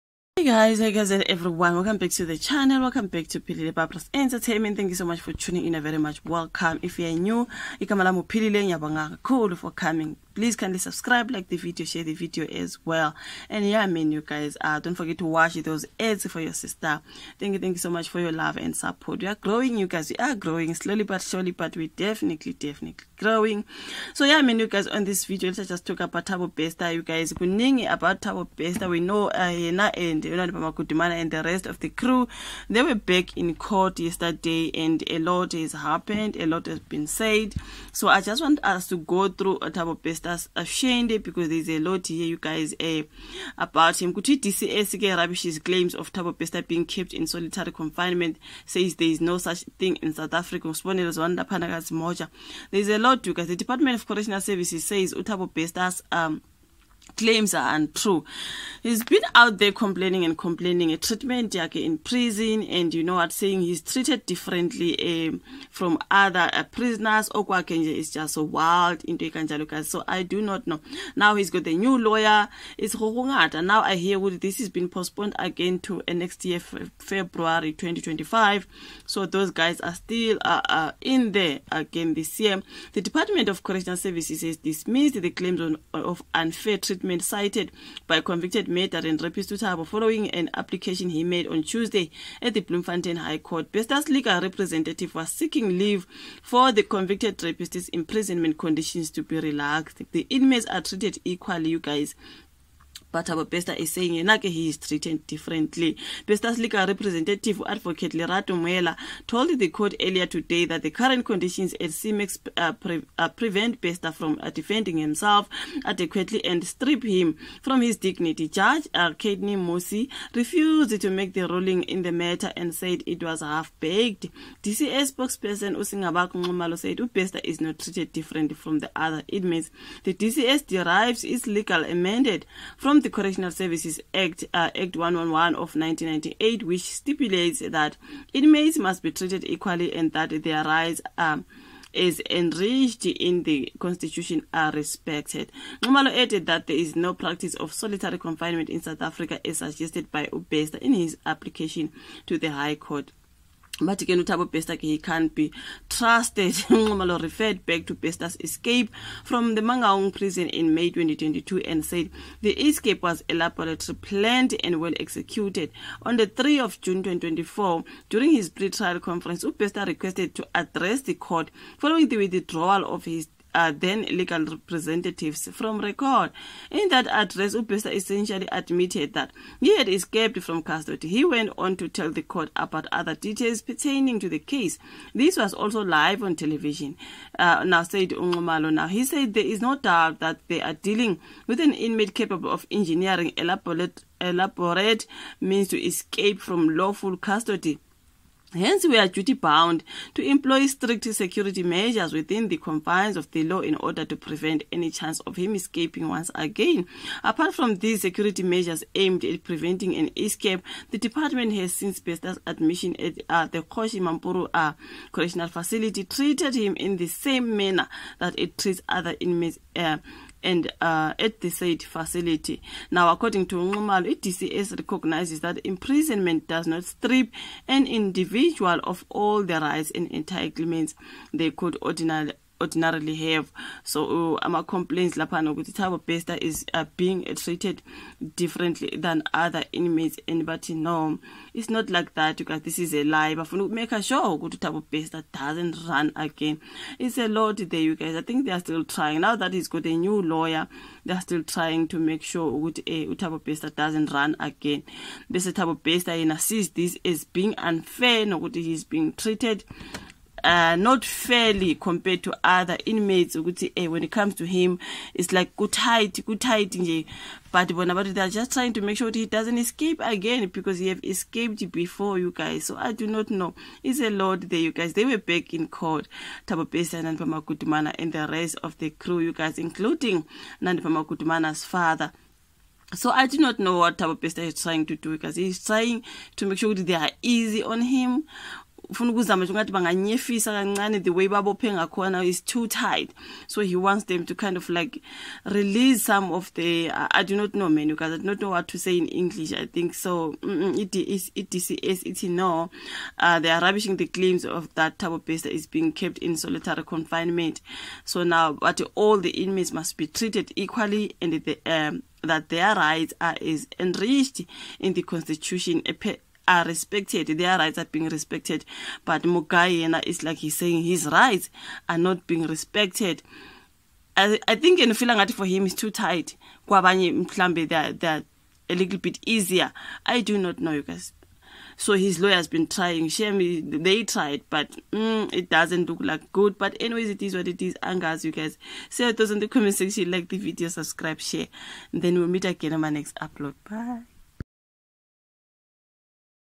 Department of Correctional Services says Thabo Bester isn't kept in solitary confinement
DCS rubishes the claims that Thabo Bester is being kept in solitary confinement#thabobester#prisonescape#prisonlivesmatter